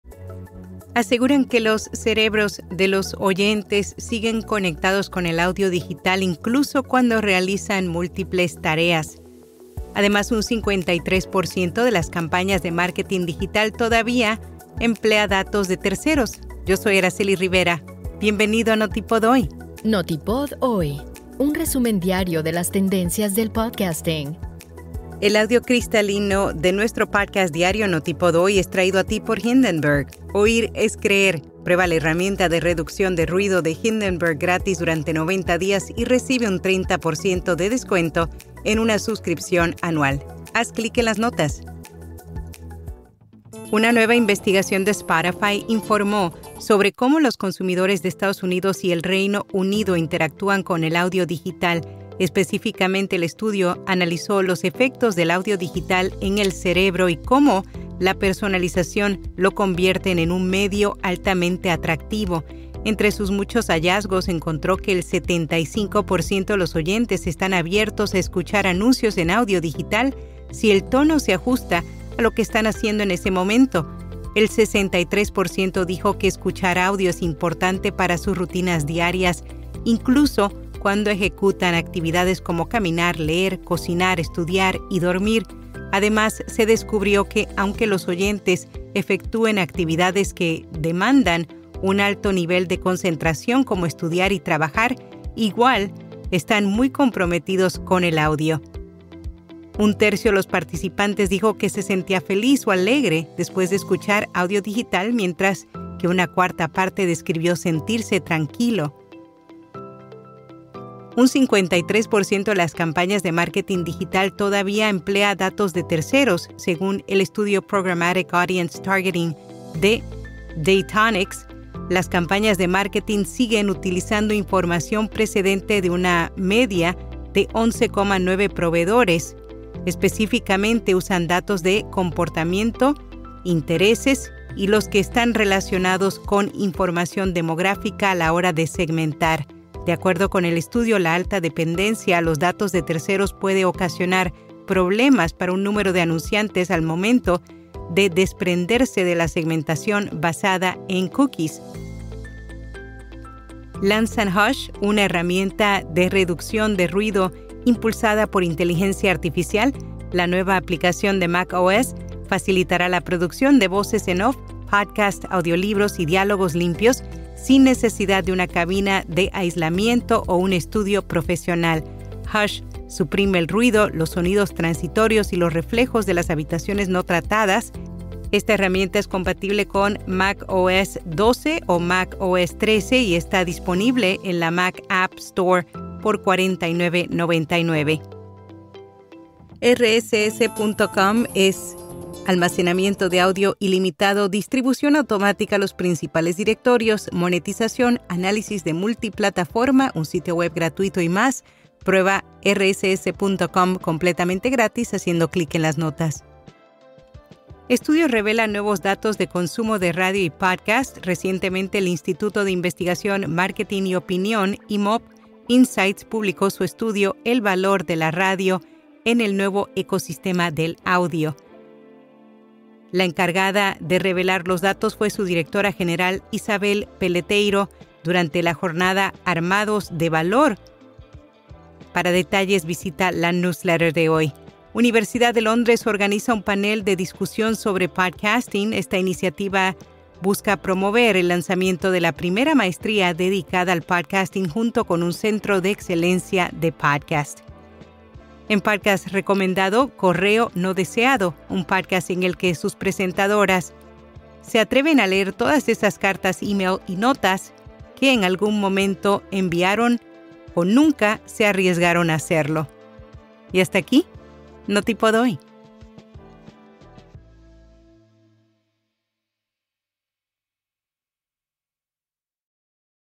Lanzan herramienta de IA para reducir ruidos cuando se graba en una habitación imperfecta
Lo que está cambiando el podcasting y el marketing digital:-Aseguran que los cerebros de los oyentes siguen conectados con el audio digital incluso cuando realizan múltiples tareas.-Un 53 % de las campañas de marketing digital todavía emplea datos de terceros.-Lanzan Hush, una herramienta de reducción de ruido impulsada por IA.-Estudio revela nuevos datos de consumo de radio y pódcast.-Universidad de Londres organiza un panel de discusión sobre podcasting.Pódcast recomendadoCorreo no deseado. Un pódcast en el que sus presentadoras Malena Guinzburg y Connie Ballarini, se atreven a leer todas esas cartas, emails y notas que en algún momento enviaron o nunca se arriesgaron a hacerlo. Cartas de amor y desamor que quedaron en el olvido y qué episodio tras episodio traen de vuelta para transformar toda esa nostalgia en muchas risas.Patrocinado por Hindenburg (el Software que usamos para editar nuestro pódcast) y Rss.com (compañía de alojamiento de pódcast)